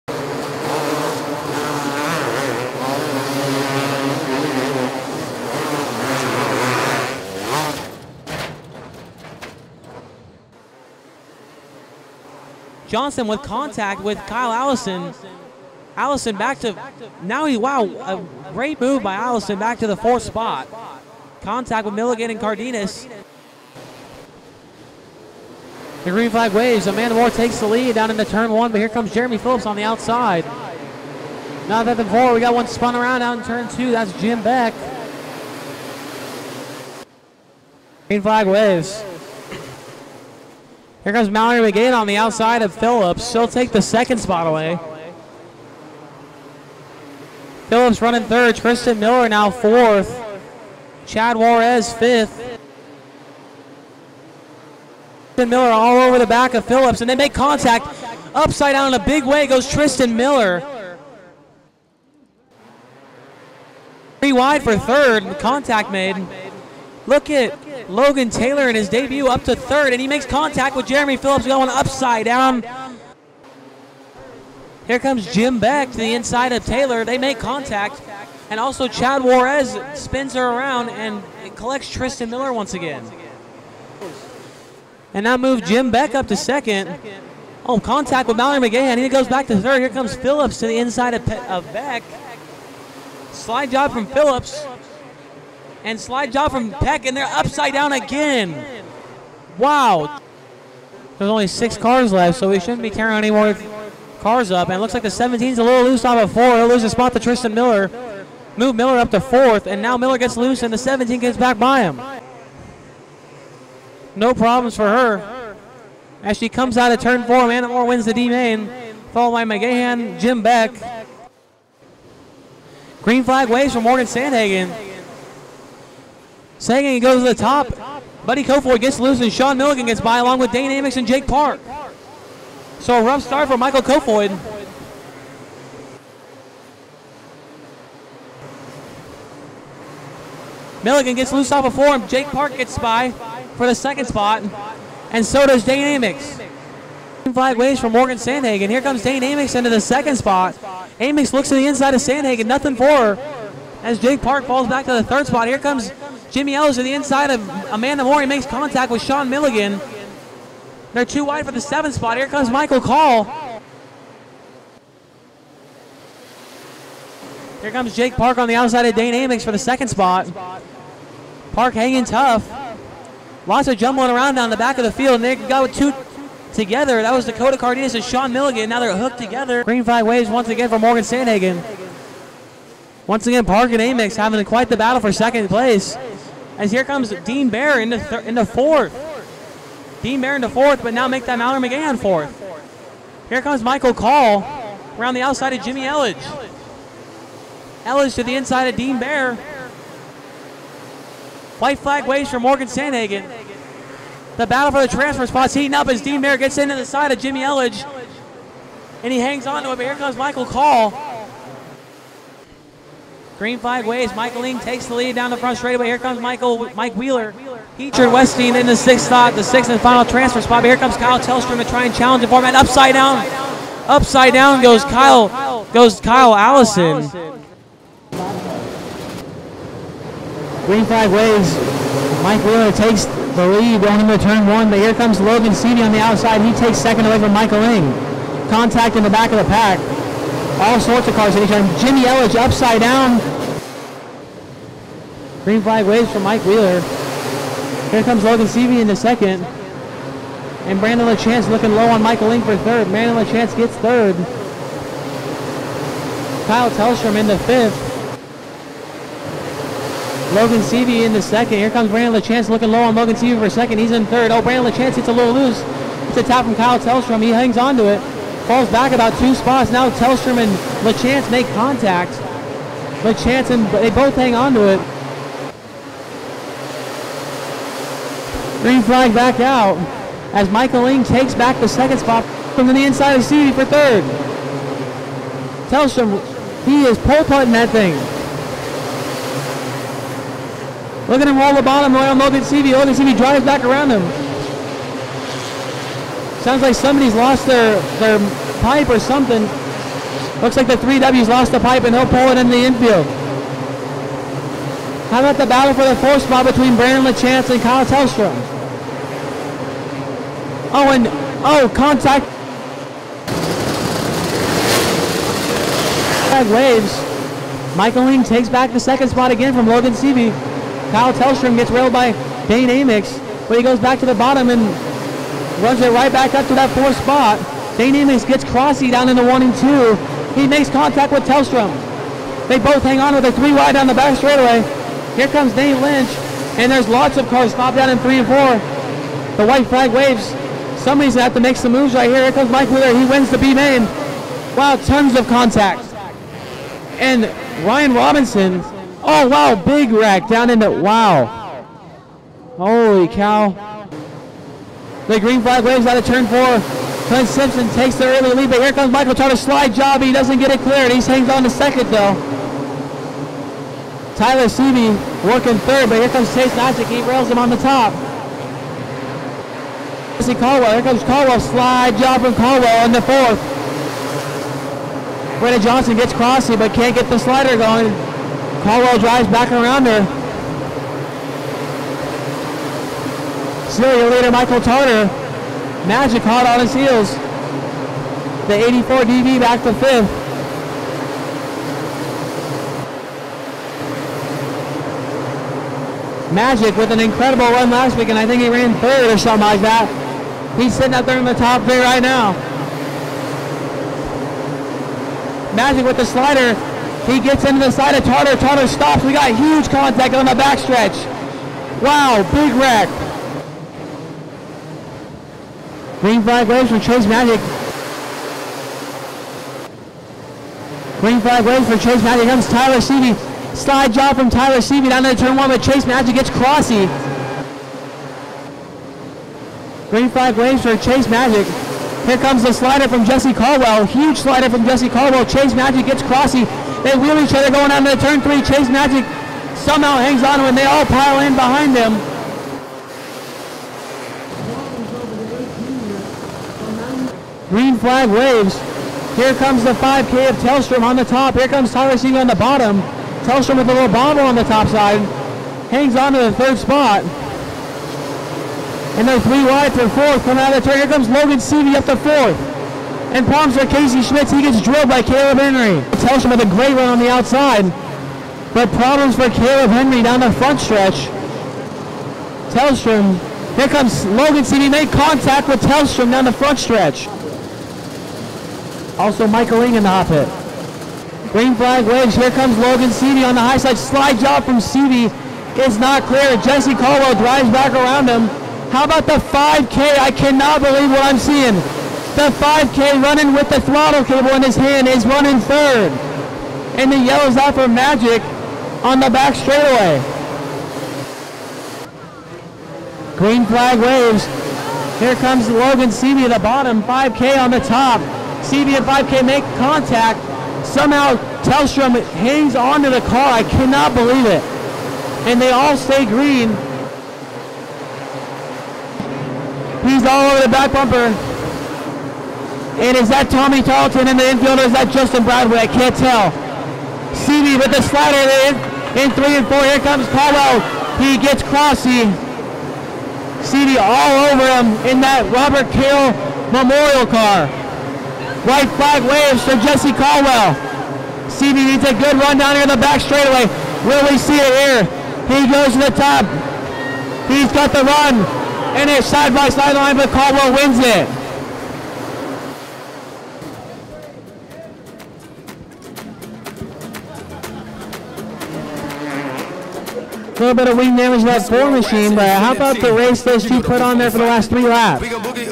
500 Open 1/10/15
Highlights of the 500 Open Division during Round #1 of the Winter Outlaw Challenge Series.